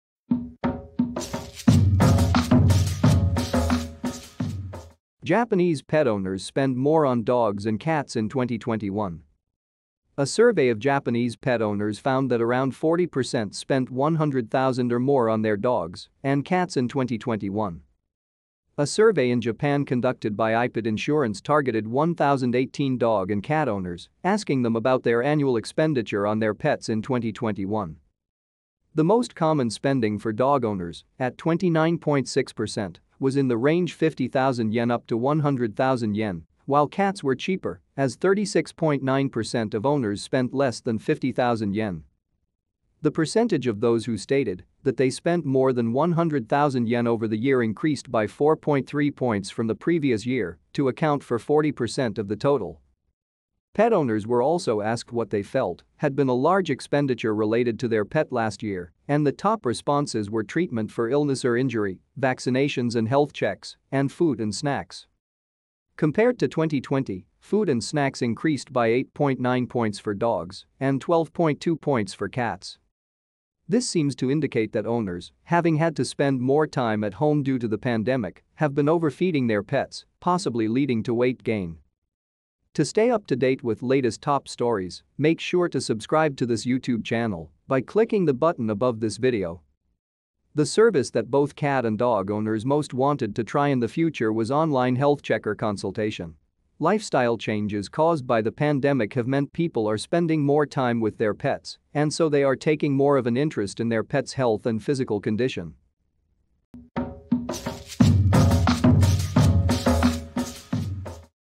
Japanese Pet Owners Spend More on Dogs and Cats in 2021
A survey of Japanese pet owners found that around 40% spent?100,000 or more on their dogs and cats in 2021.
A survey in Japan conducted by ipet Insurance targeted 1,018 dog and cat owners, asking them about their annual expenditure on their pets in 2021.
The most common spending for dog owners, at 29.6%, was in the range ¥50,000 up to ¥100,000, while cats were cheaper, as 36.9% of owners spent less than ¥50,000.
The percentage of those who stated that they spent more than ¥100,000 over the year increased by 4.3 points from the previous year to account for 40% of the total.
Pet owners were also asked what they felt had been a large expenditure related to their pet last year and the top responses were “treatment for illness or injury,” “vaccinations and health checks,” and “food and snacks”.
Compared to 2020, “food and snacks” increased by 8.9 points for dogs and 12.2 points for cats.
This seems to indicate that owners, having had to spend more time at home due to the pandemic, have been overfeeding their pets, possibly leading to weight gain.